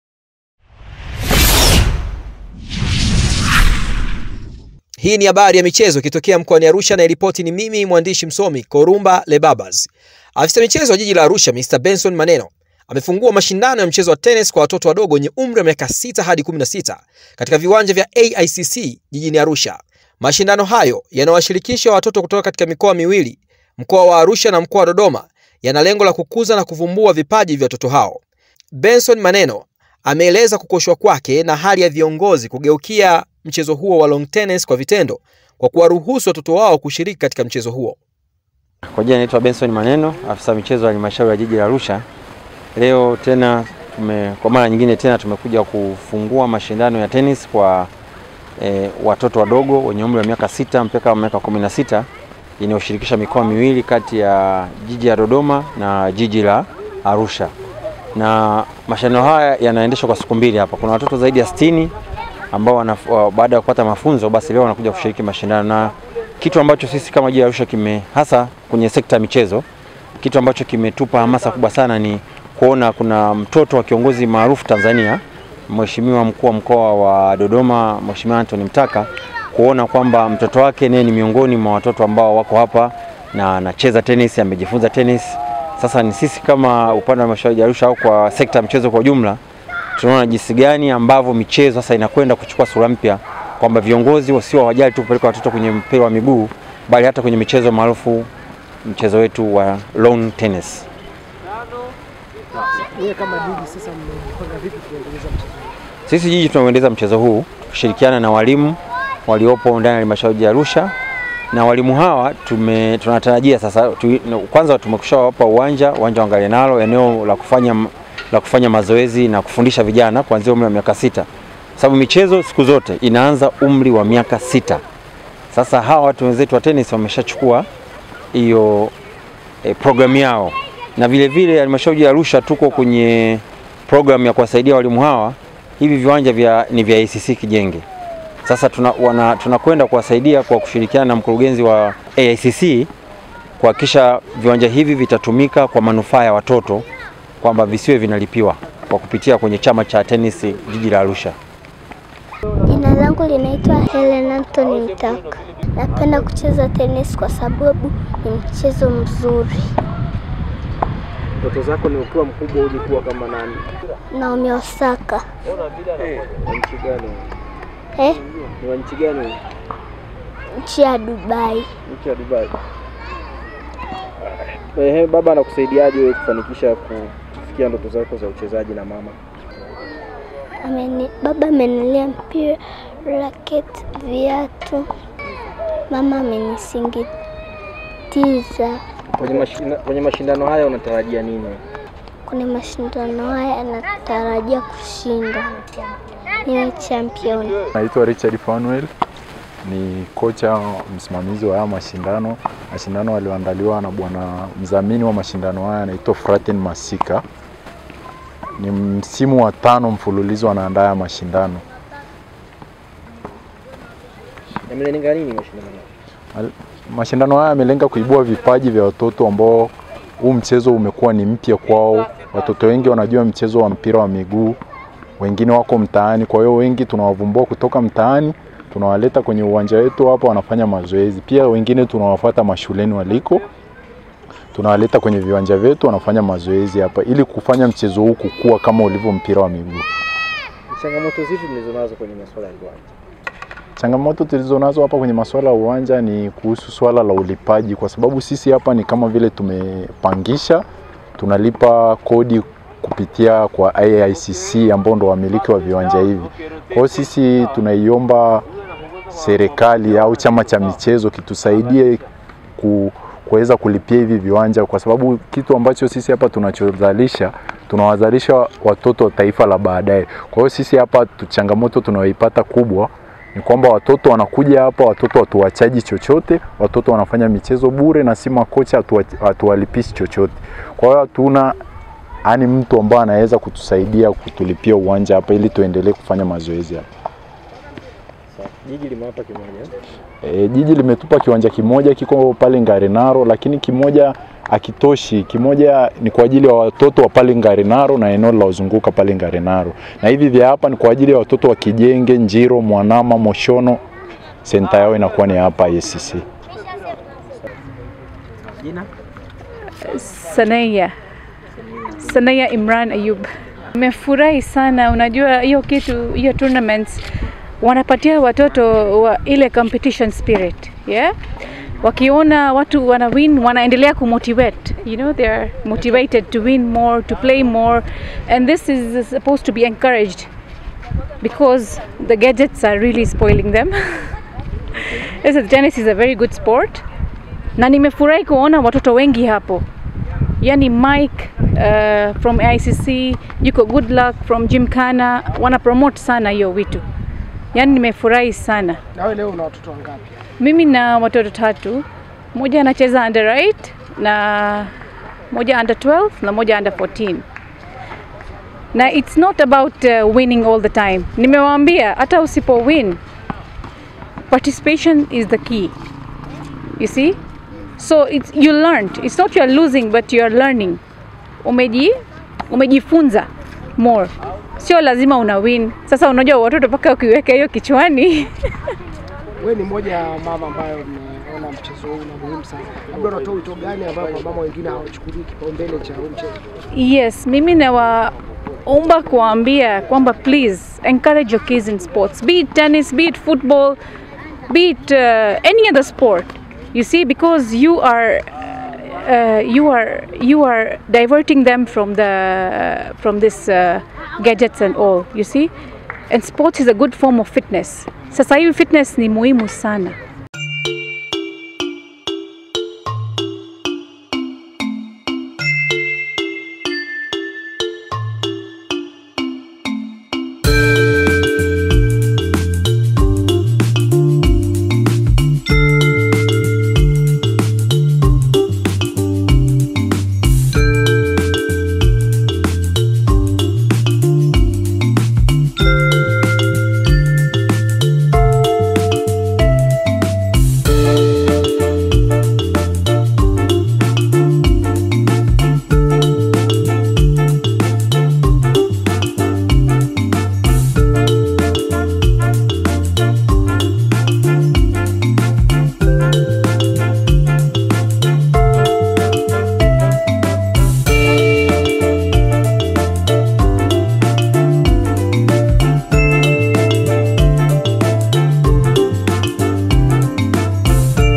TAZAMA MTOTO wa RC MTAKA ANAVYOCHEZA TENISI, ANA KIPAJI BALAA, Aongea - "NATAKA KUWA NAOMI OSAKA"
Afisa michezo wa jiji la Arusha Mr Benson Maneno, amefungua mashindano ya mchezo wa tenisi kwa watoto wadogo wenye umri wa mika 6 hadi miaka 16 katika viwanja vya Aicc jijini Arusha...

Mashindano hayo yanashirikisha watoto kutoka  mikoa miwili Mkoa wa Arusha na Dodoma yenye lengo la kukuza vipaji vya watoto hao.